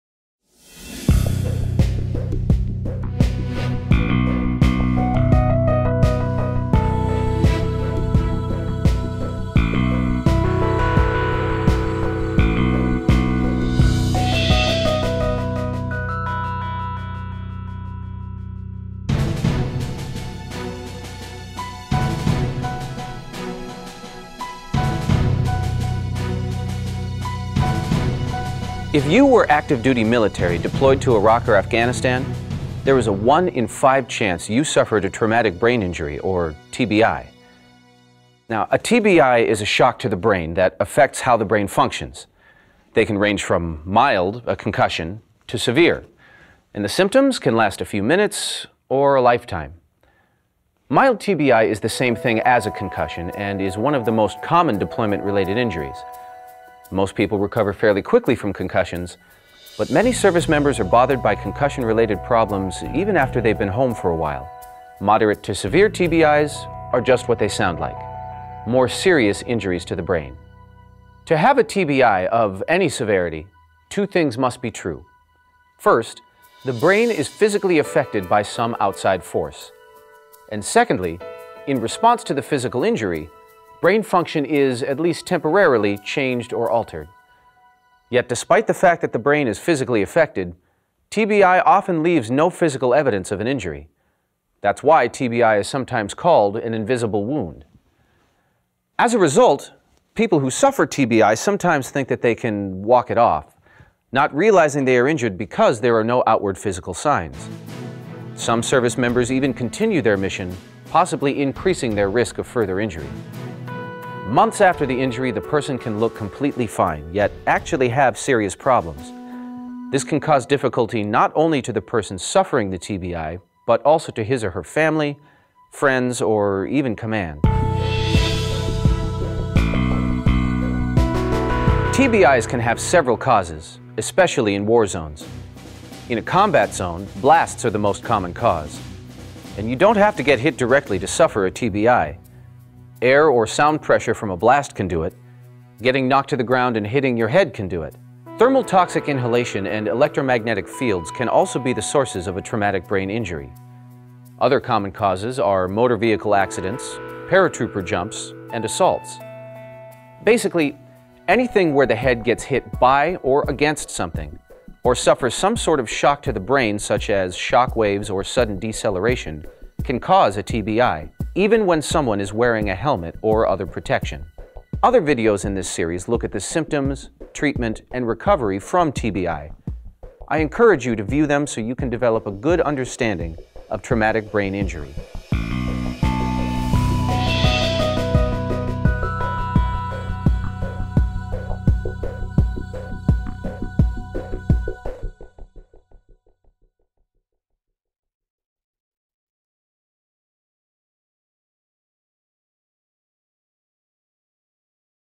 What is TBI and how is it caused?
What is traumatic brain injury and what are some of the common causes.